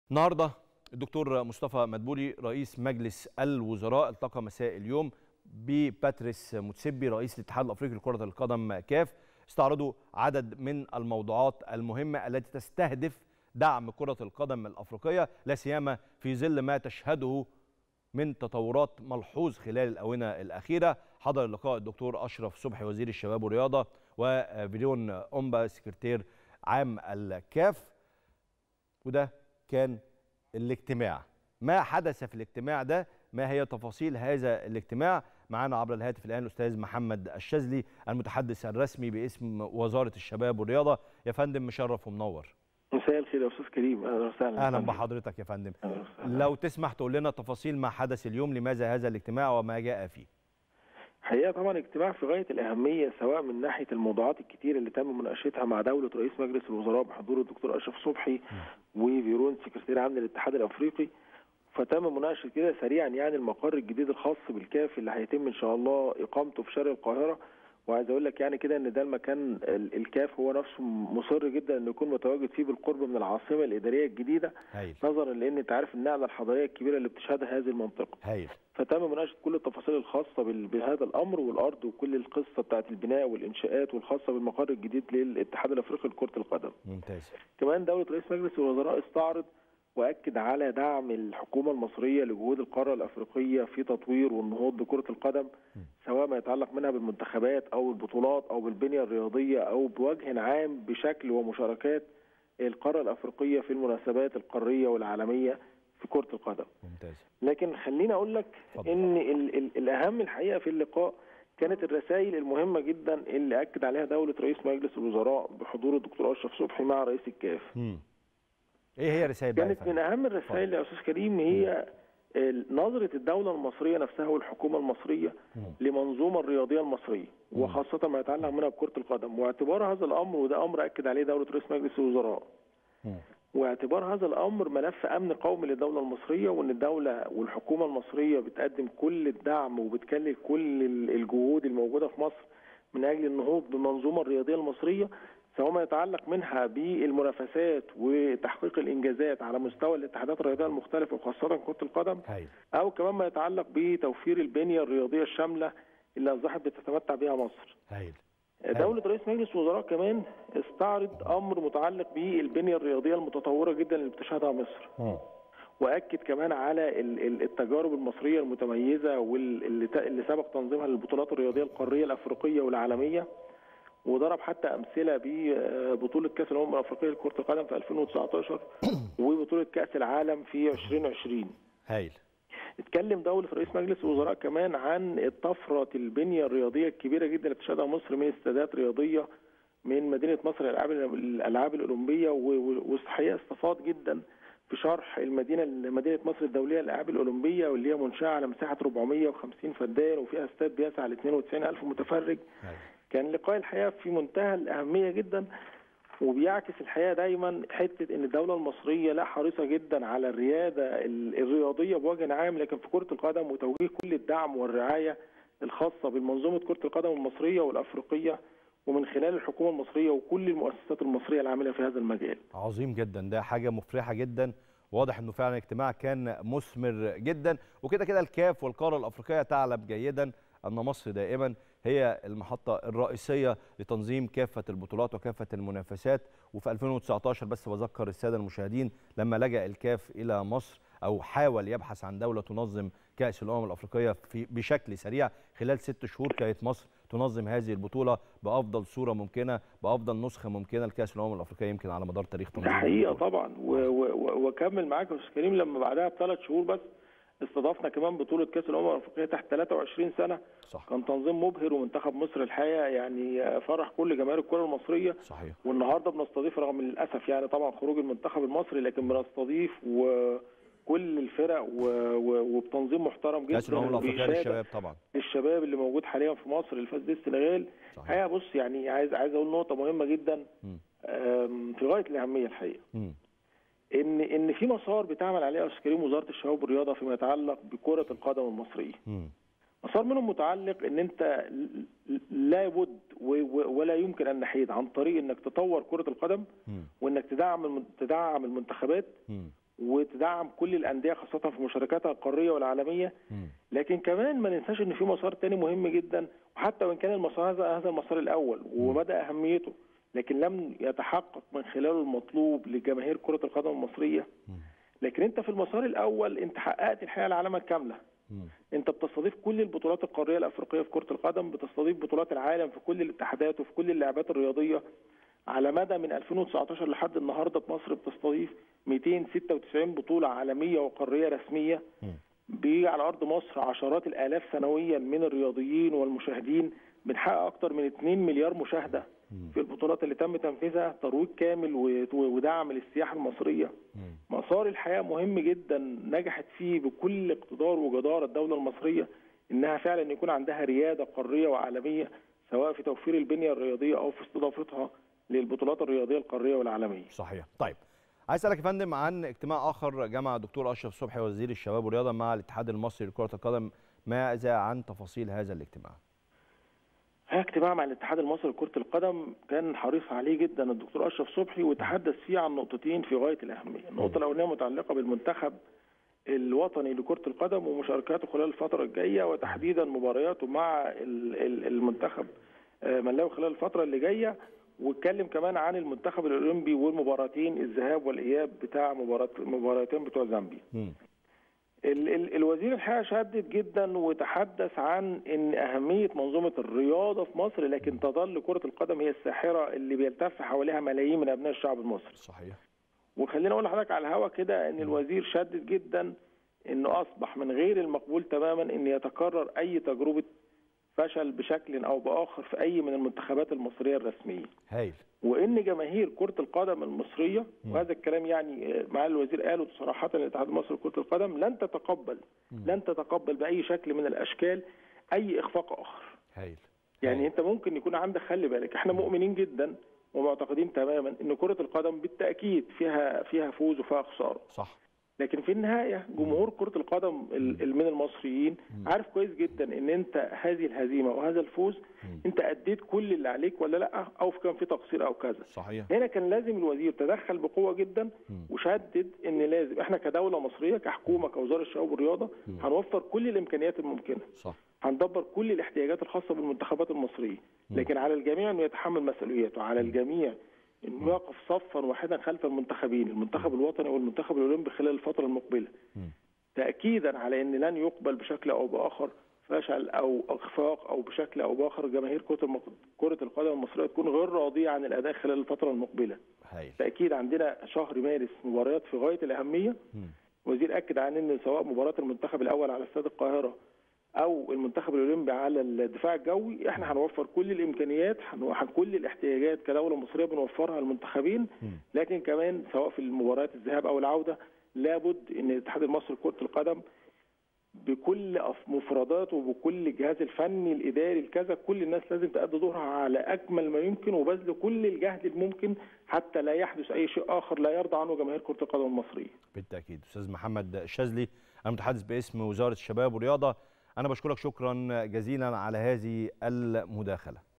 🔥🔥المتحدث باسم وزارة الشباب والرياضة يكشف تفاصيل اجتماع رئيس الوزراء مع رئيس اتحاد الكرة المصري
"لان نتحمل او نتقبل اي اخفاق آخر" 🔥🔥المتحدث باسم وزارة الشباب والرياضة يكشف تفاصيل اجتماع رئيس الوزراء مع رئيس اتحاد الكرة المصري